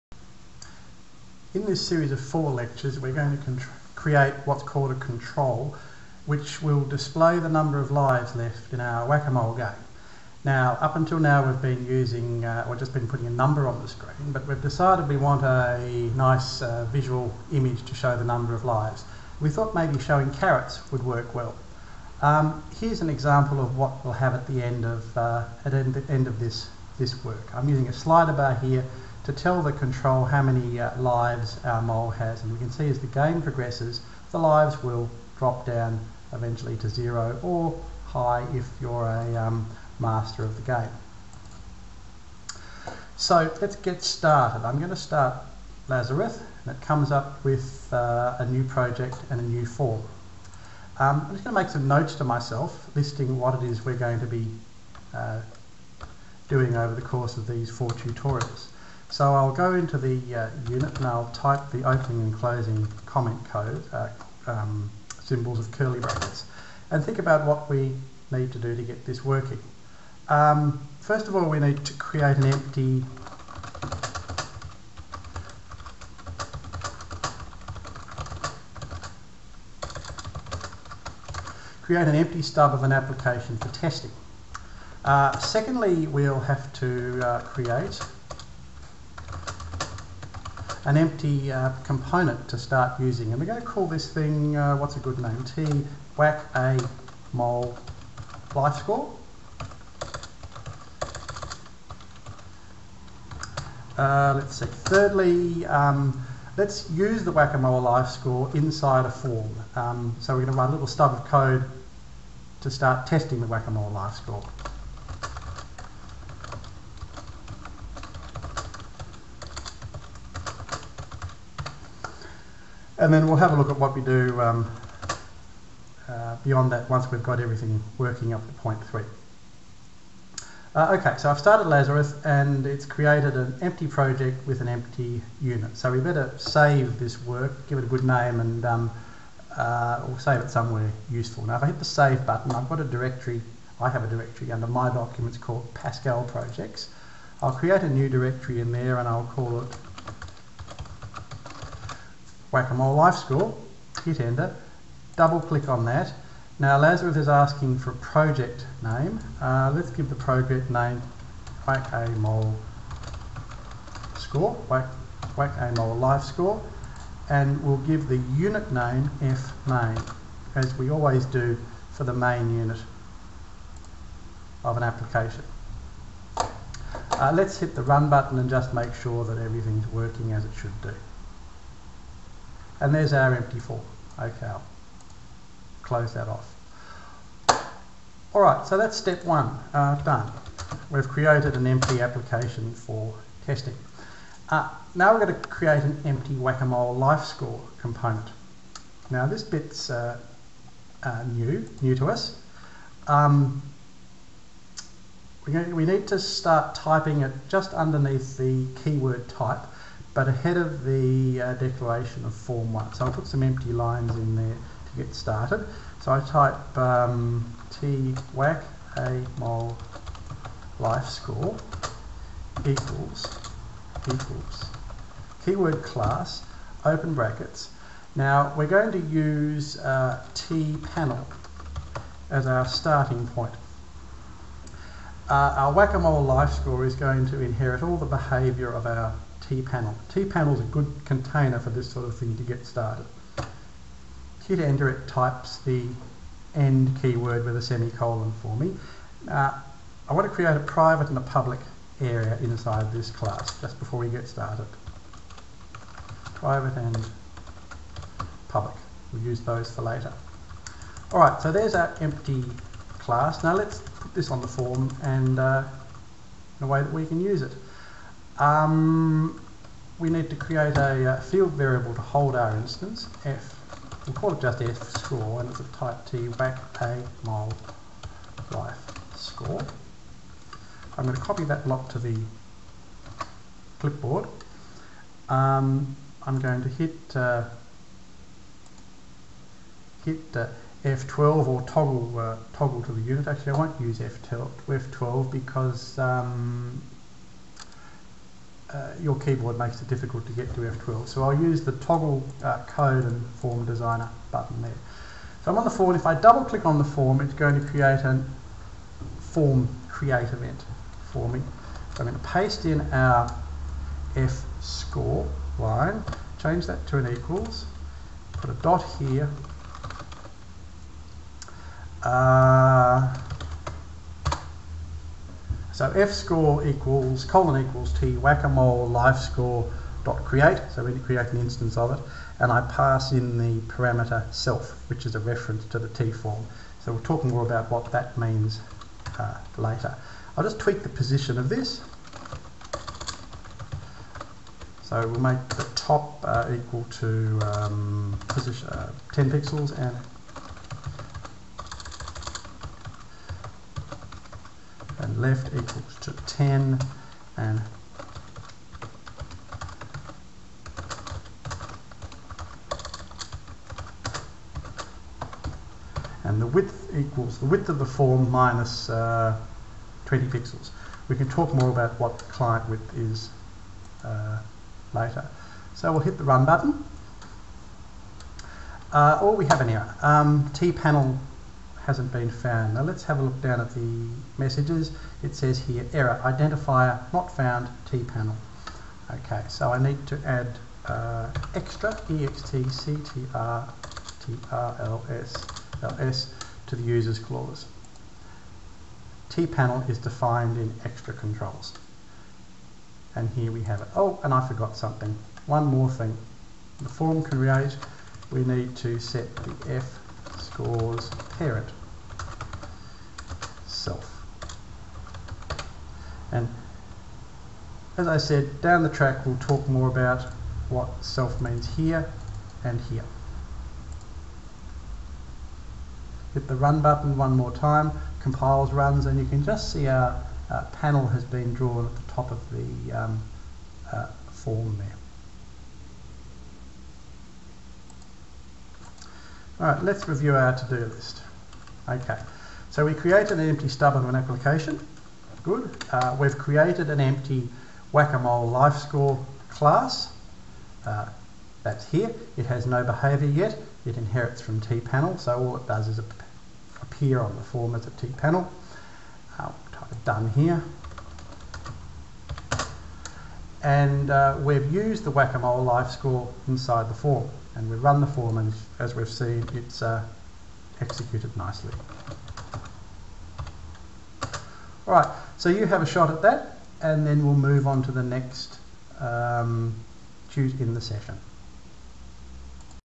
ClubEngineer Write a Game in Pascal Create a Score Component #1
Code a Game in Pascal - Create a Score Component
Tutorial 1/4

* Create an empty stub of an application to work with;
* Create an empty stub of the TWAMLifeScore;
* Use the TWAMLifeScore component inside an application for testing;